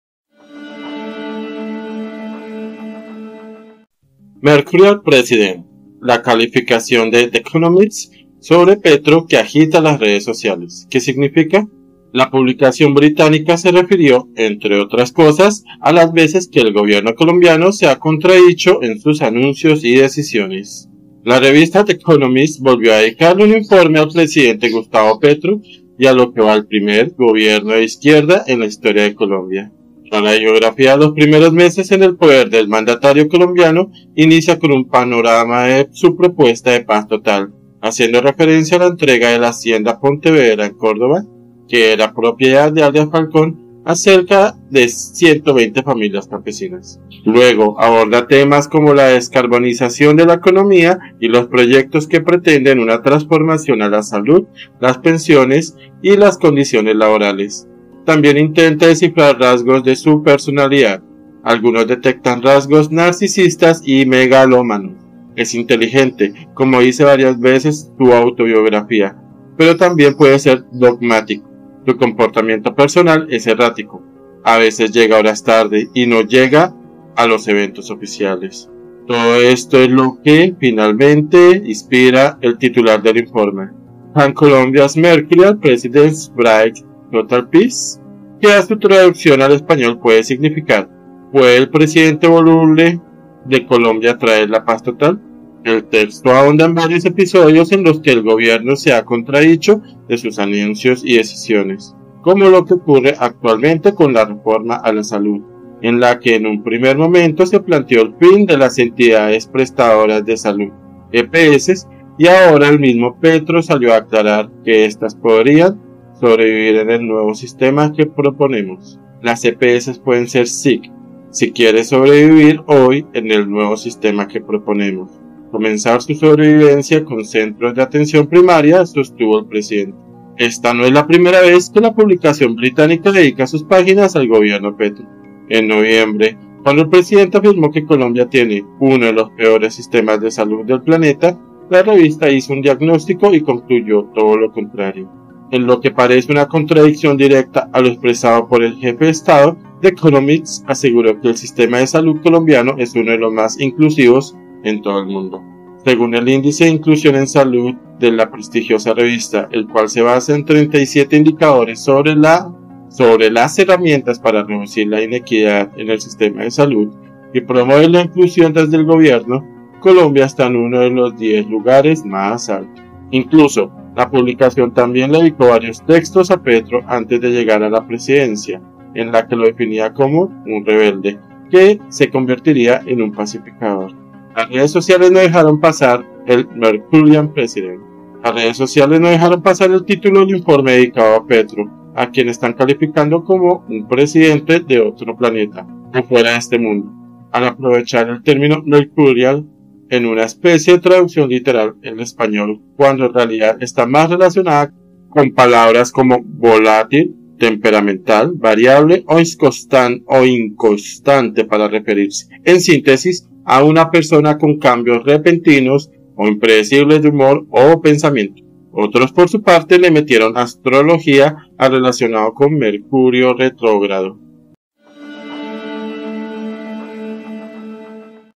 EL PRESIDENTE VOLUBLE 🔴 Fuerte Columna The Economist sobre Petro
La revista The Economist volvió a dedicarle un informe al presidente Gustavo Petro y a lo que va del primer Gobierno de izquierda en la historia de Colombia.